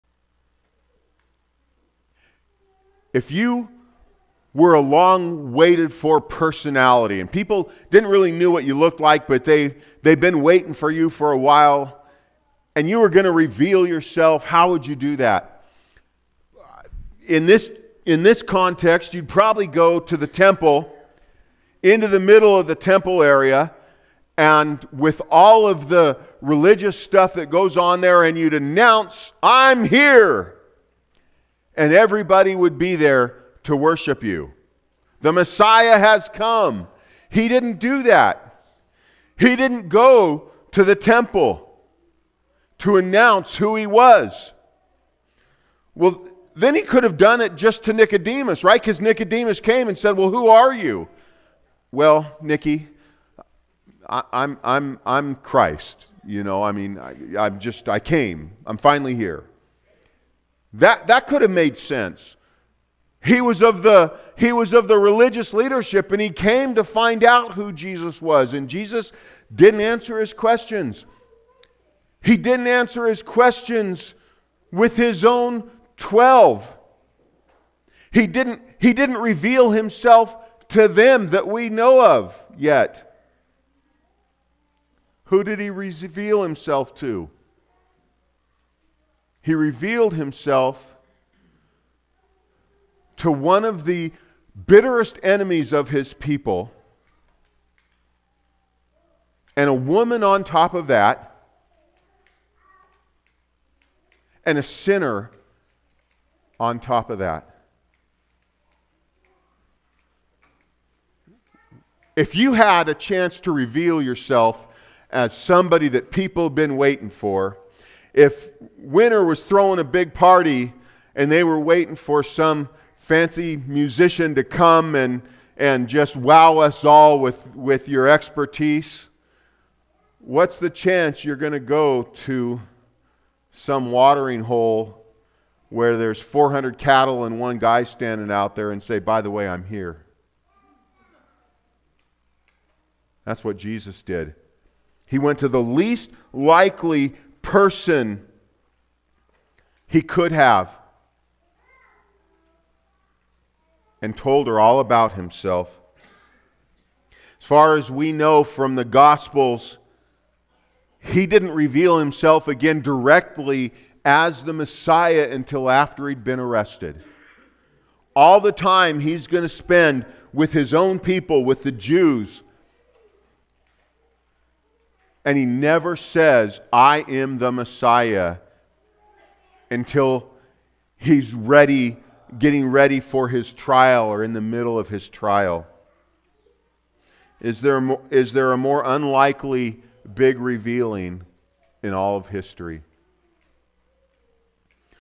To a Samaritan Woman?
If you were going to do a premier showing, a "big reveal", what audience would you choose?

This is a short clip from Pastor Tony's message from John 4:16-26.